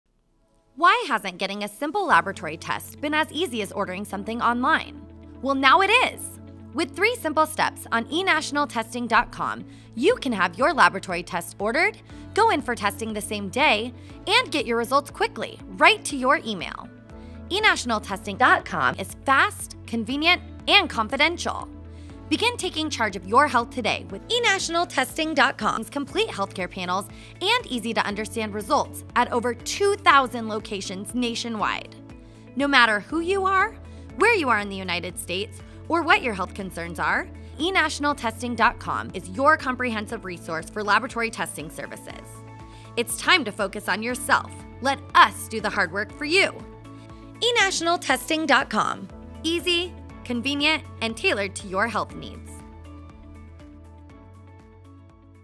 eNational Testing - What We Do.
No matter who you are, where you are, or what your health concerns are, eNational Testing is your comprehensive resource for laboratory testing services. Through our secure platform, we offer a complete range of testing services. Take charge of your health today with eNational Testing’s complete diagnostic panels and easy-to-understand results, available at over 2,000 locations nationwide!